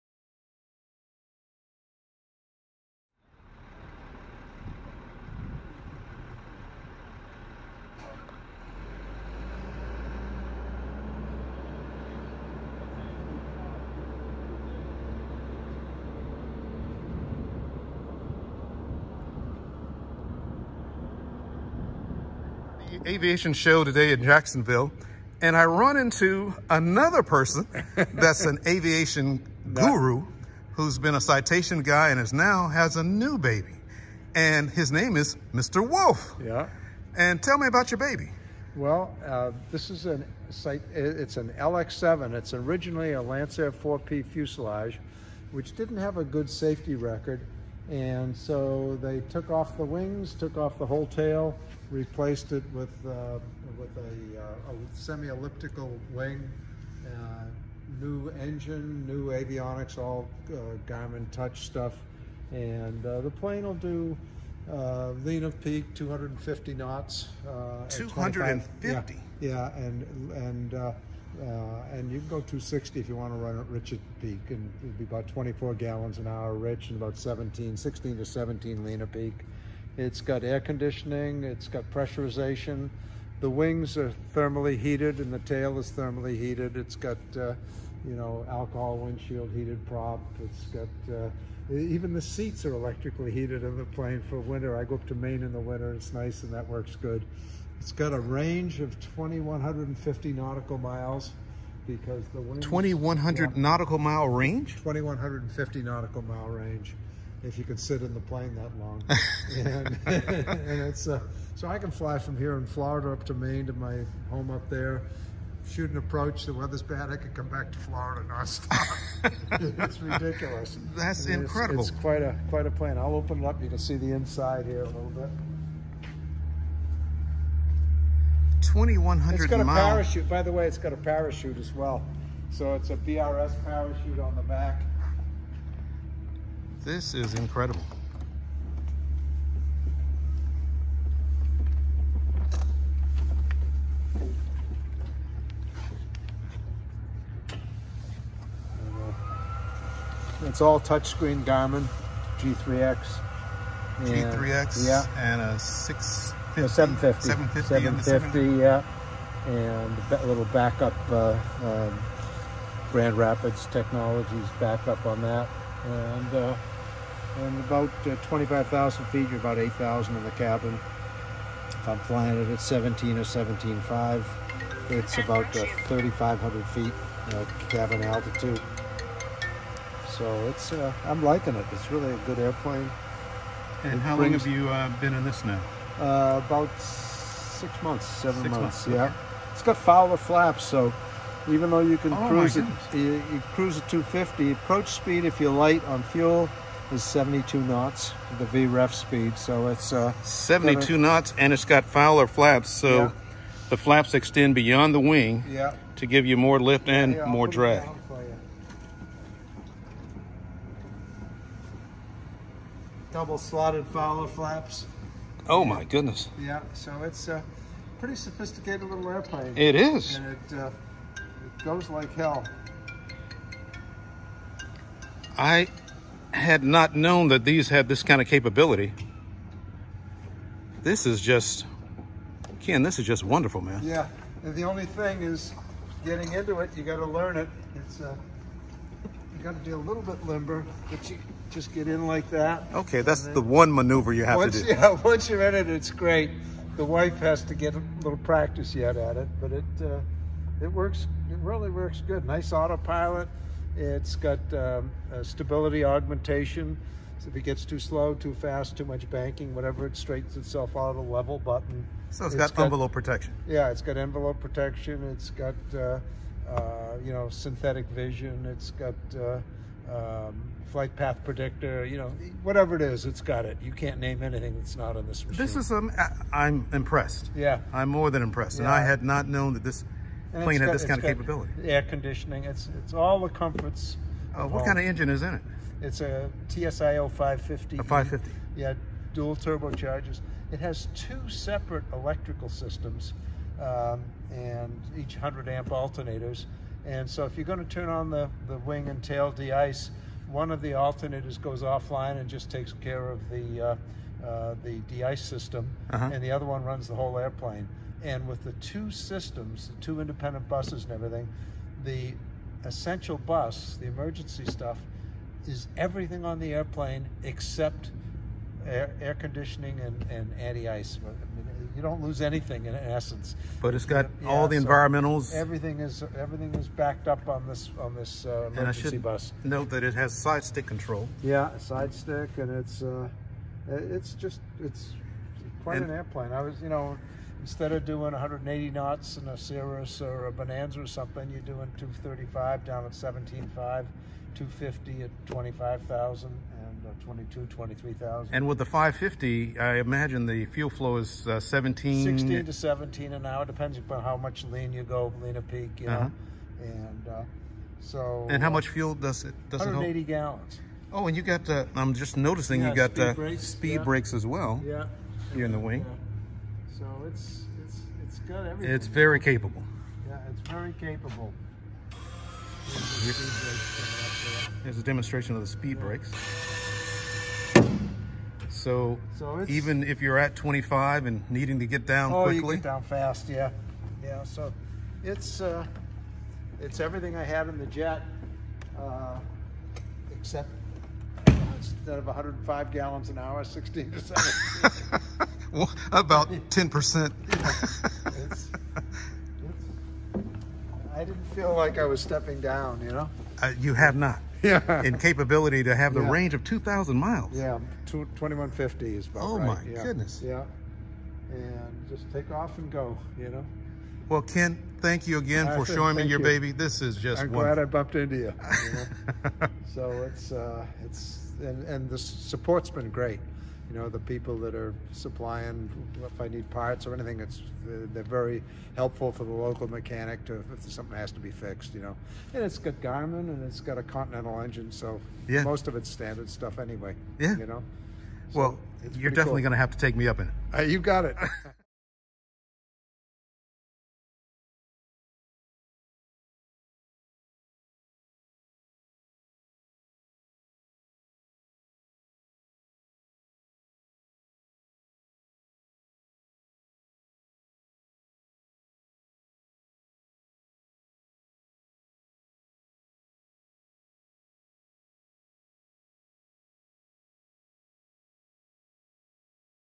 LX7 Review with Ken Wolf @ KVQQ Feb 2020
A short review of the Lancair LX7 with Ken Wolf (not from Pulp Fiction LOL). We meet during the CLIMB event which provides an early introduction of aviation to elementary and secondary kids in Jacksonville. The event was held at Cecil Field (KVQQ) in Jacksonville, Florida.

FROM RDD Enterprises:
The LX7 is the foundational model of the LX7 series of aircraft. This model is a piston powered, pressurized cabin environment, retractable tricycle landing gear, high performance aircraft. The LX7 was born out of a love for high cruise speed and safe slow speed handling characteristics. The Continental TSIO-550-E engine produces 350 Horsepower for take-off and climb, directing that power smoothly to a metal, scimitar Hartzell constant speed propeller. A pressurized cabin is key to reducing fatigue of the pilot and passengers while enabling flight at higher altitudes with a greater degree of safety. Pilot centric workload reducing features abound in the LX7 as the safety of the occupants is the highest goal of the design, from dual-redundant main spar structures in the wing, to a whole aircraft parachute system, to standard airframe deicing equipment, the LX7 incorporates everything you may want plus many things you need to reduce workload and enable the pilot to make better decisions. The instrument panel contains an envelope protecting autopilot with an upset recovery function and large displays for situational awareness. Each of the four seats is appointed with heaters for the bases and the backs to provide additional comfort for everyone. The large windows accommodate a great view from the pressurized capsule that is capable of transcontinental flights, so you can see the sights from San Franciso to Washington D.C. non-stop.